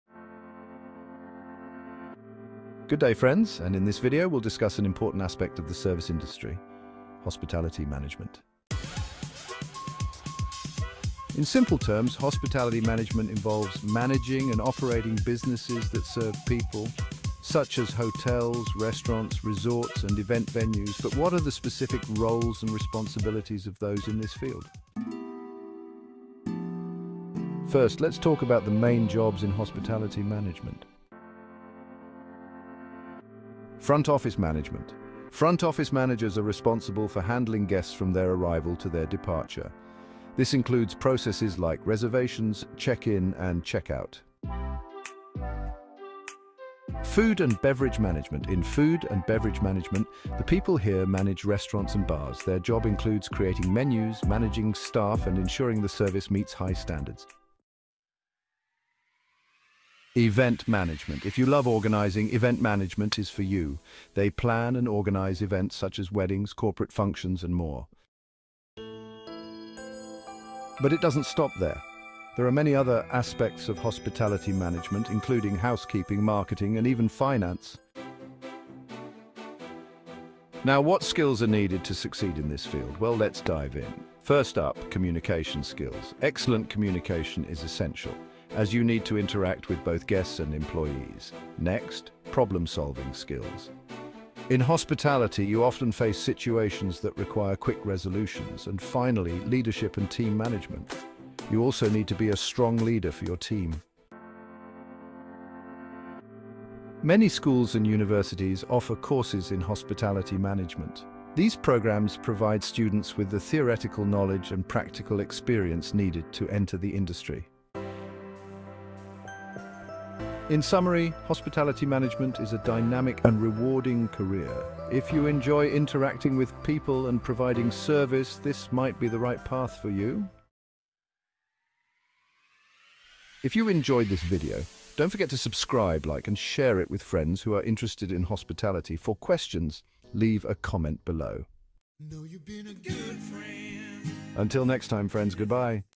Ano Ang Trabaho ng Hospitality Management
Sa video na ito, tatalakayin natin ang iba't ibang trabaho sa larangan ng Hospitality Management. Alamin kung ano ang mga tungkulin at responsibilidad ng mga propesyonal sa industriya ng hotel, restaurant, at iba pang hospitality services. Mula sa front desk operations hanggang sa culinary arts, ipapakita natin ang mga career paths na maaari mong tahakin. Kung ikaw ay interesado sa isang masiglang karera sa hospitality, huwag palampasin ang mga impormasyong ito!

Mga Topic na Tatalakayin:
- Pangkalahatang ideya ng hospitality management
- Mga trabaho at responsibilidad sa industriya
- Mga kasanayan na kailangan sa hospitality
- Career opportunities at growth in hospitality